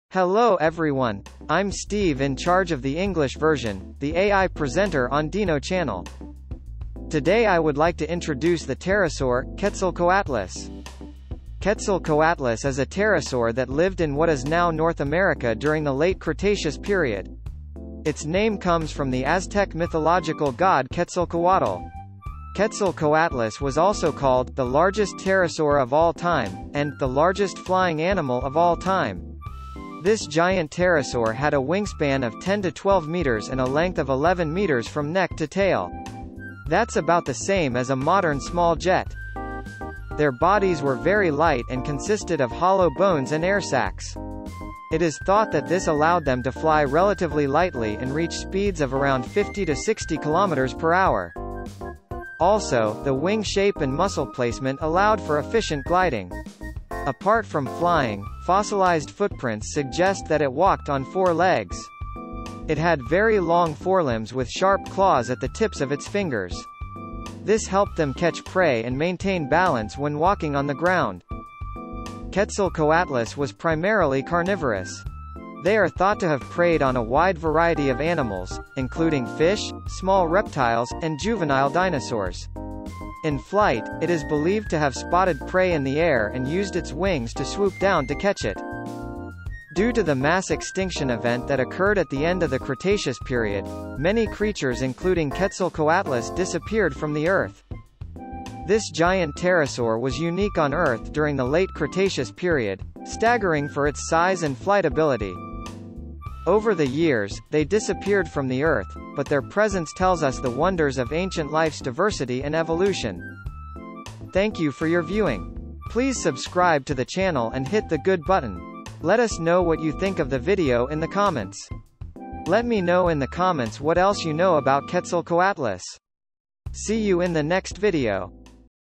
Largest pterosaur!!【 Quetzalcoatlus 】Short Dinosaur Explanation!《:DINOchannnel English ver.》
I will explain the dinosaur commentary in a short time!
Short Dinosaur Explanation
22th「Quetzalcoatlus/ケツァルコアトルス」

If you have any Quetzalcoatlus anecdotes, please let me know!